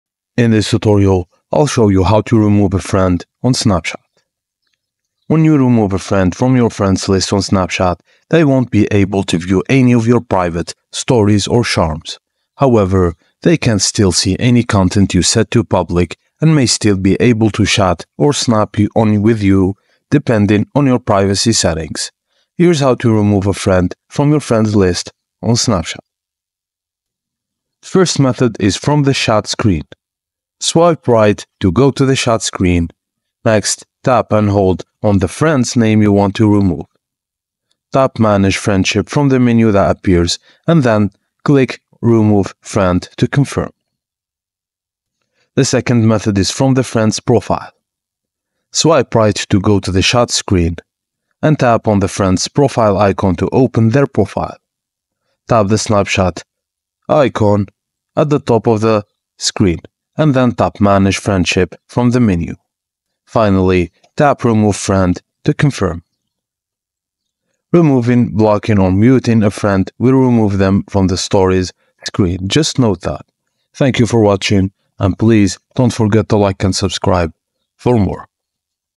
How to Delete Friends on Snapchat (2024 Guide)
Welcome to our 2024 guide on how to delete friends on Snapchat! In this video, we’ll walk you through the simple steps to remove someone from your friends list. Whether you’re cleaning up your contacts or want to manage your friends list more effectively, this tutorial is for you.

🔹 Steps Covered in the Video:
Opening Snapchat
Accessing Your Friends List
Finding the Friend to Delete
Opening Their Profile
Accessing More Options
Deleting the Friend